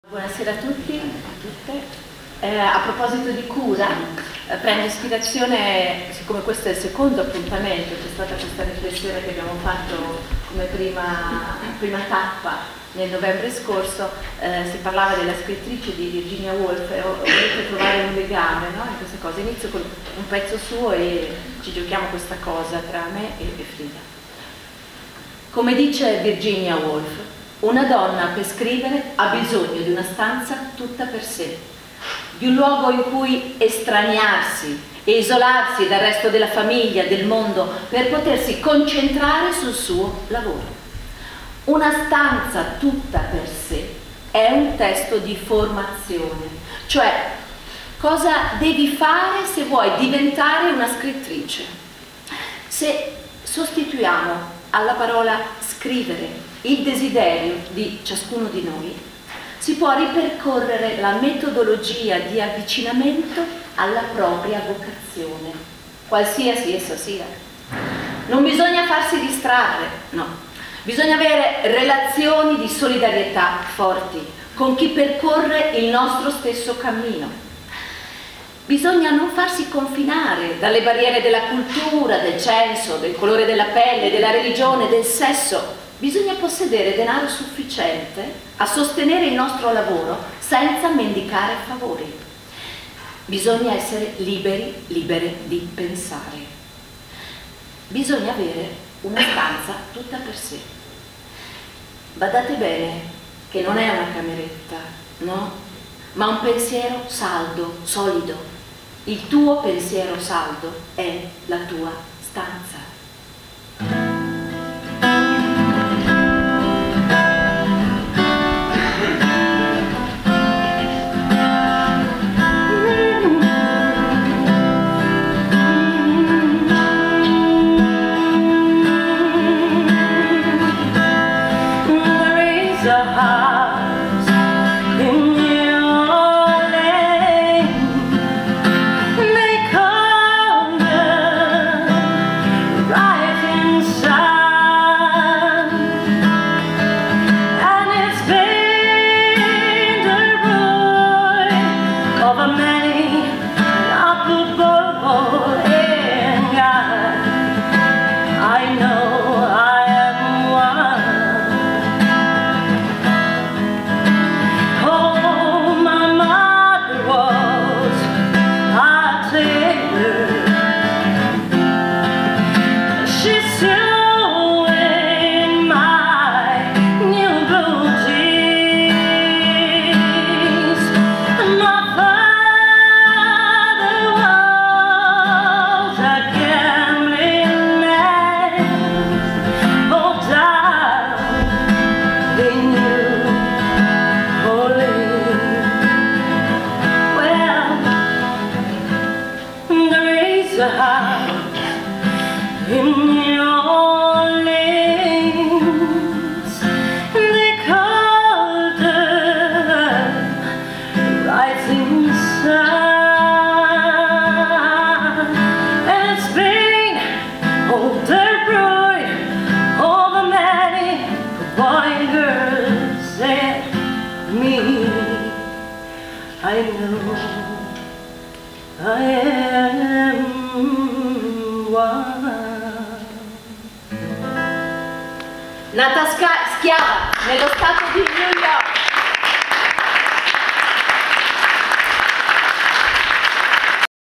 Roberta Biagiarelli legge Virginia Woolf "Cosa serve a una donna per scrivere"
4 marzo 2015 Donne in sicurezza. La lotta al crimine parte da noi stesse, organizzata dall'Assessorato alle Pari Opportunità del Comune di Fano, è stato preceduto da una lettura di Roberta Biagiarelli e da un assolo di Frida Neri che ha cantato un pezzo di Mercedes Sosa